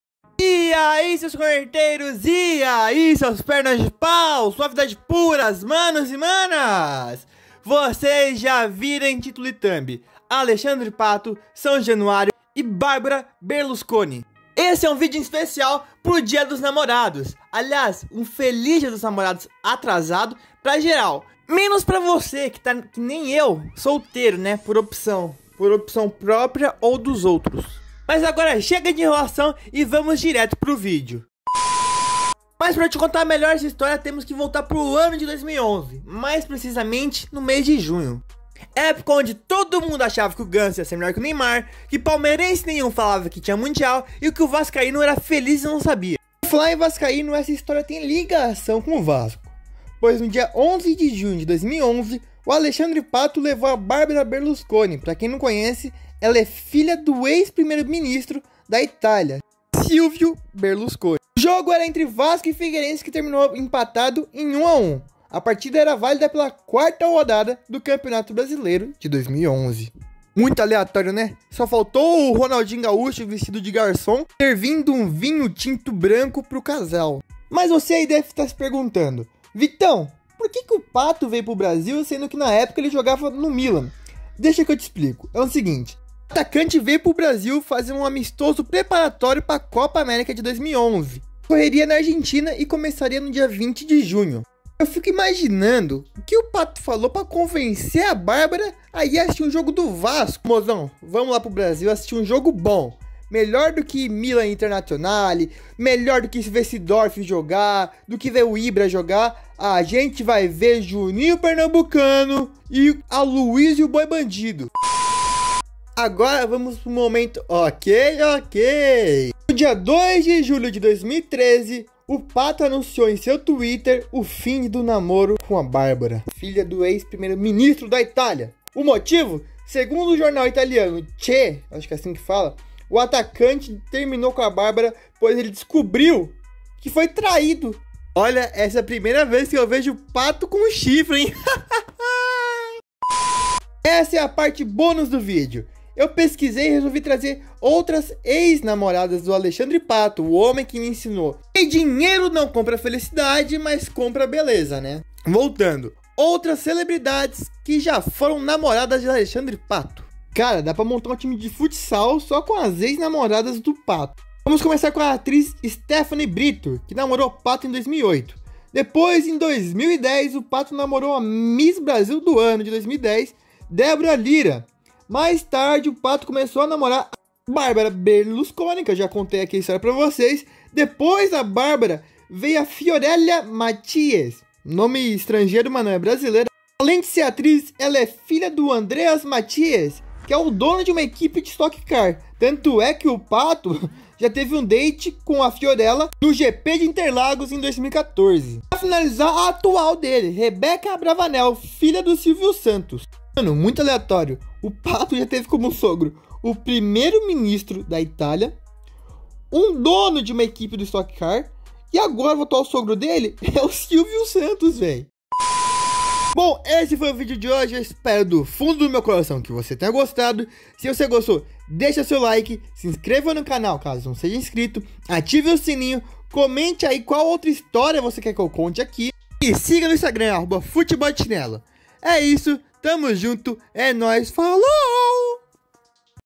O dia em que Alexandre Pato levou a Barbara Berlusconi pra um jantar romântico no São Januário
Pato coleciona ex-namoradas celebridades, uma delas é a Barbara Berlusconi, filha do primeiro ministro da Itália. O atacante levou a Barbara pra assistir o jogo entre Vasco e Figueirense no Brasileirão de 2011. Resolvi fazer um vídeo, explicando melhor tudo isso e falando outras celebridades que Alexandre Pato já namorou.

E comente qual história (sobre futebol) curiosa, você quer que eu conte.

TMJ